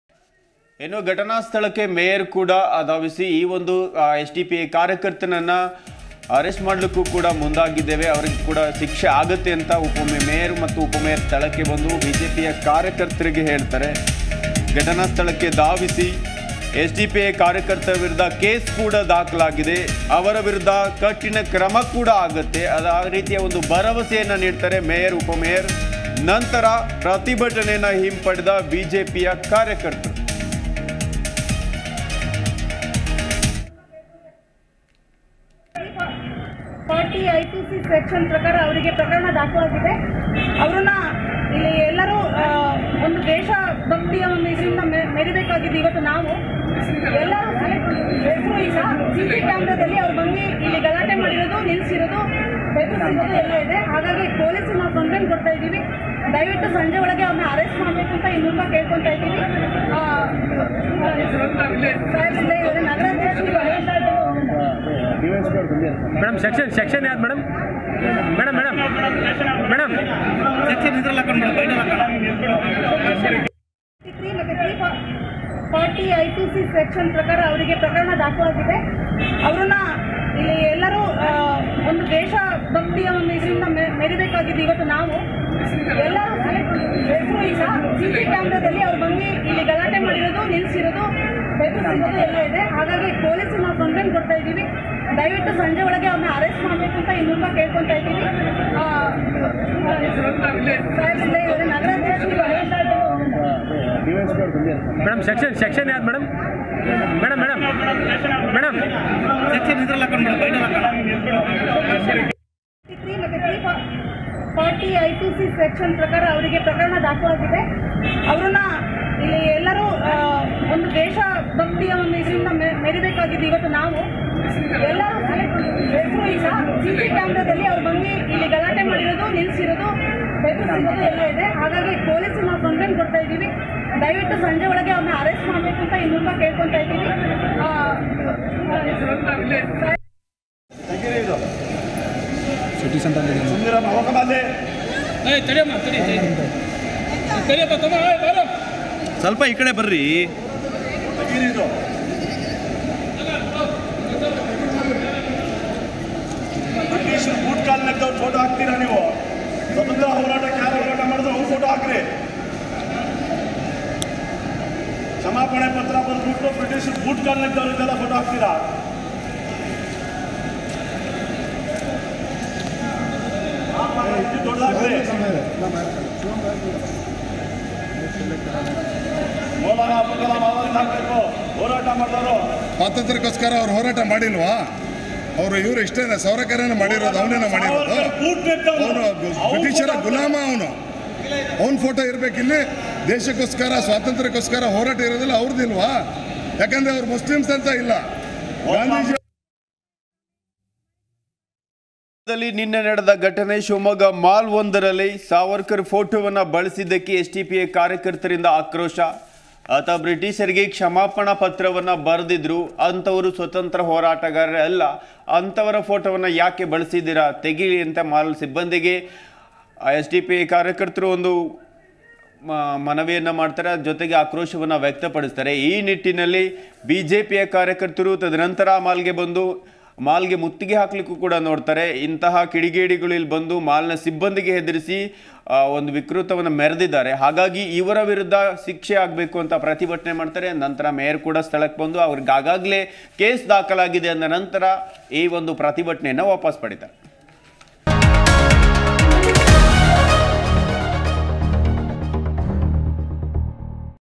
MAYOR VISIT || ಸೂಕ್ತ ಕ್ರಮದ ಭರವಸೆ ನೀಡಿದ ಶಿವಮೊಗ್ಗ ಮೇಯರ್
ಎಸ್‌ಡಿಪಿಐ ವಿರುದ್ಧ ಬಿಜೆಪಿ ಪ್ರತಿಭಟನೆ ನಡೆಸುತ್ತಿದ್ದ ಸ್ಥಳಕ್ಕೆ ಬಂದ ಶಿವಮೊಗ್ಗ ಮೇಯರ್ ಹಾಗೂ ಉಪಮೇಯರ್​, ಎಸ್‌ಡಿಪಿಐ ಕಾರ್ಯಕರ್ತರ ವಿರುದ್ಧ ಕೇಸ್​ ದಾಖಲಾಗಿದೆ. ಅವರ ವಿರುದ್ಧ ಕ್ರಮವಾಗಲಿದೆ ಎಂದು ಭರವಸೆ ನೀಡಿದ್ದಾರೆ. ನಂತರ ಬಿಜೆಪಿ ಕಾರ್ಯಕರ್ತರು ಪ್ರತಿಭಟನೆ ಹಿಂಪಡೆದಿದ್ದಾರೆ.